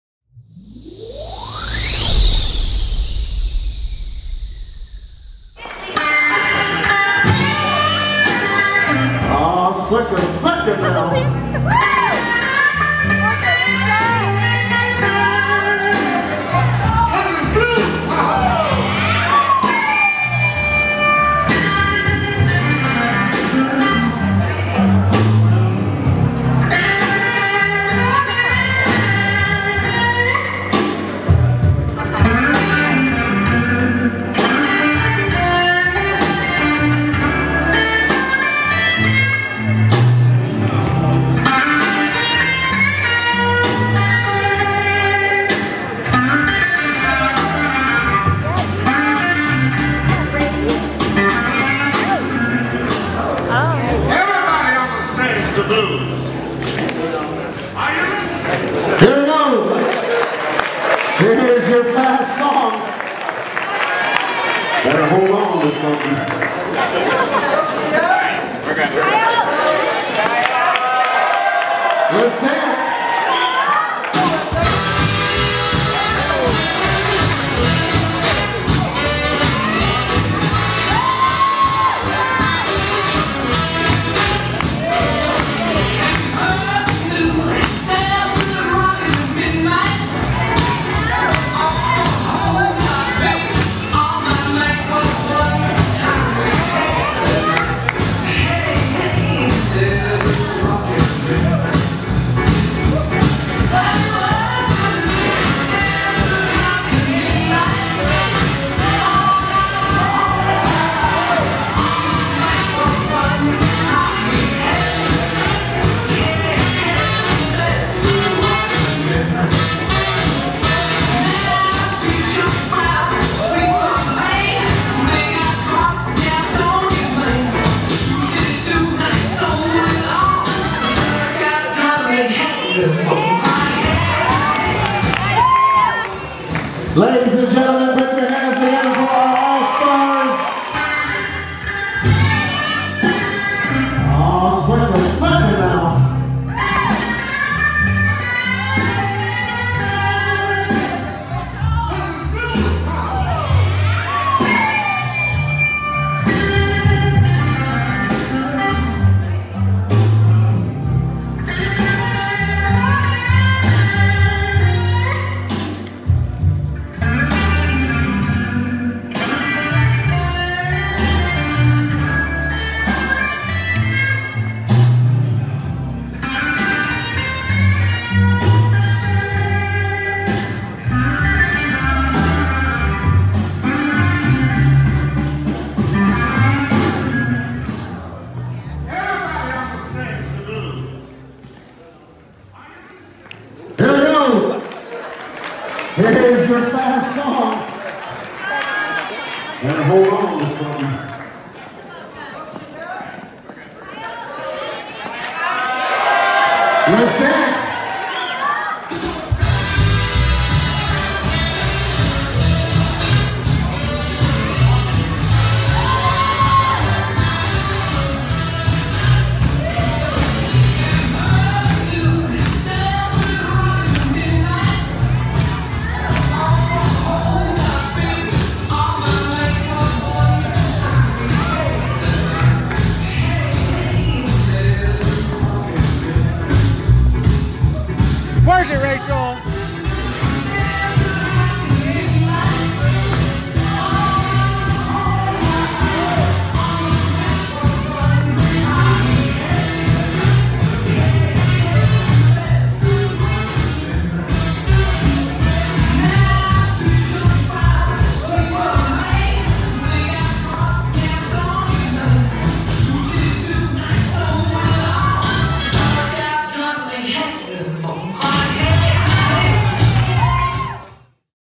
SwingTime 2018 Allstar Jack & Jill Allskate
Allstar Jack & Jill Allskate

SwingTime 2018 is an annual West Coast Swing event that took place in Denver, CO at the Hyatt Regency Aurora-Denver Conference Center from July 26th-July 30th.